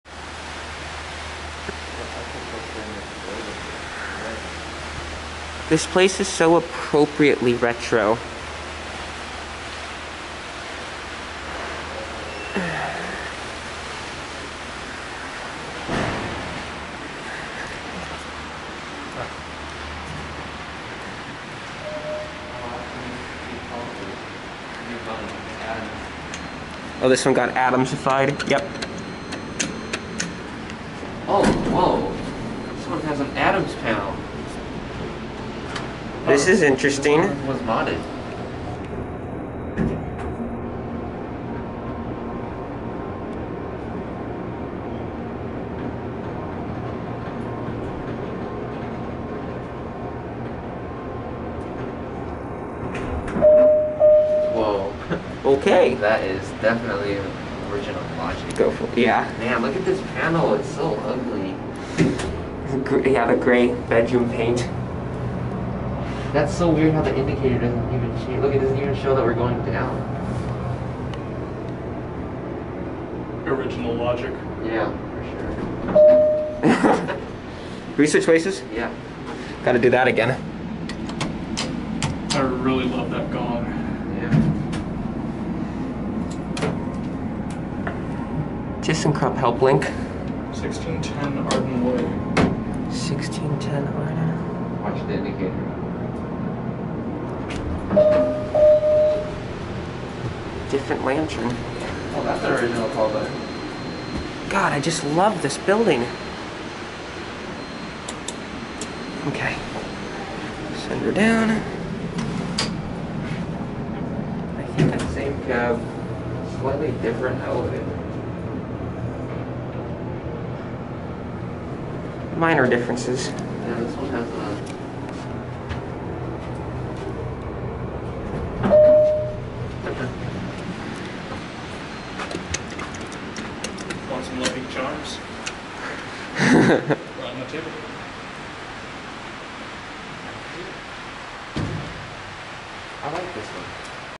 Point West Corporate Center in Sacramento Has a Second Dover Elevator
This elevator received a mod with Adams fixtures.  Otherwise, it is identical to the first elevator.  Nice cab and poor quality paint job on the metal.

Date Recorded: March 16, 2021